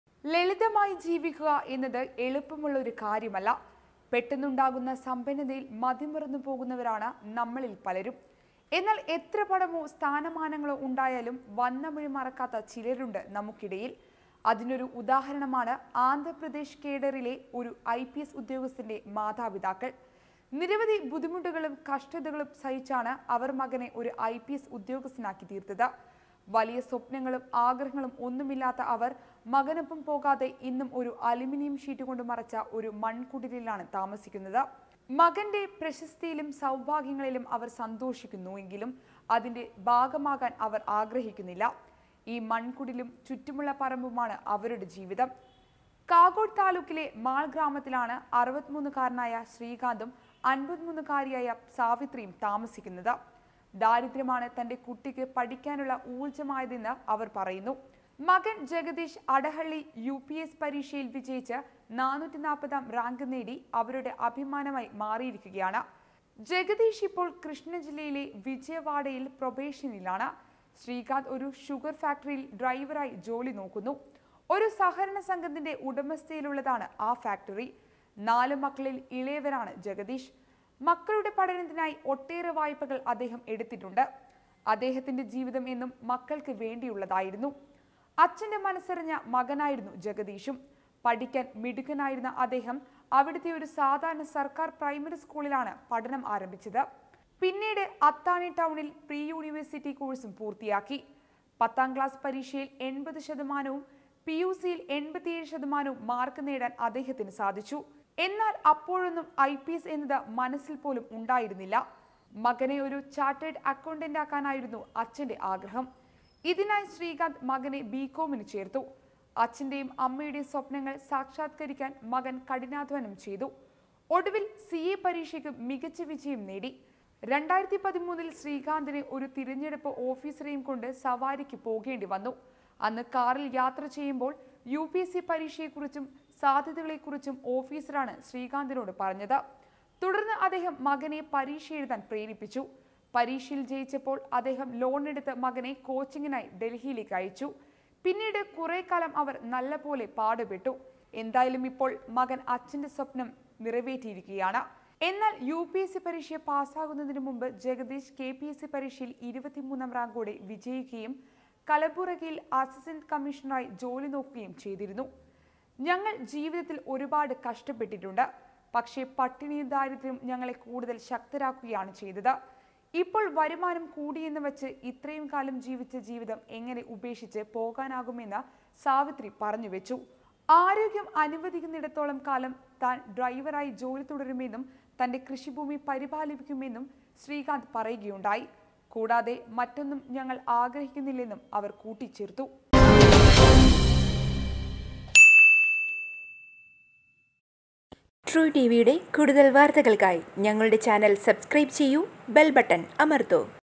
ഈ ഐപിഎസുകാരന്റെ മാതാപിതാക്കള്‍ താമസിക്കുന്നത് മണ്‍കുടിലില്‍! | life inspiration
ഈ ഐപിഎസുകാരന്റെ മാതാപിതാക്കള്‍ താമസിക്കുന്നത്  മണ്‍കുടിലില്‍!